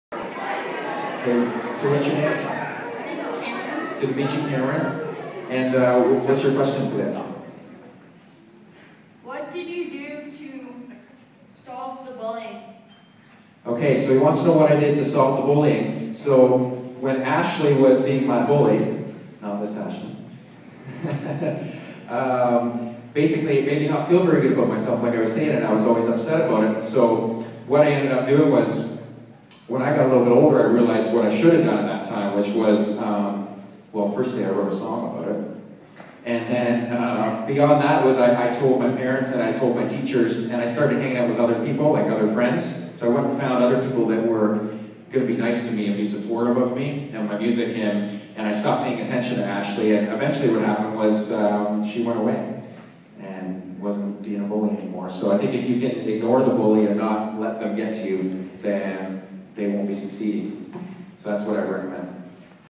Anti-Bullying Q&A with Ryan Laird
Ryan Laird answers elementary student questions about bullying.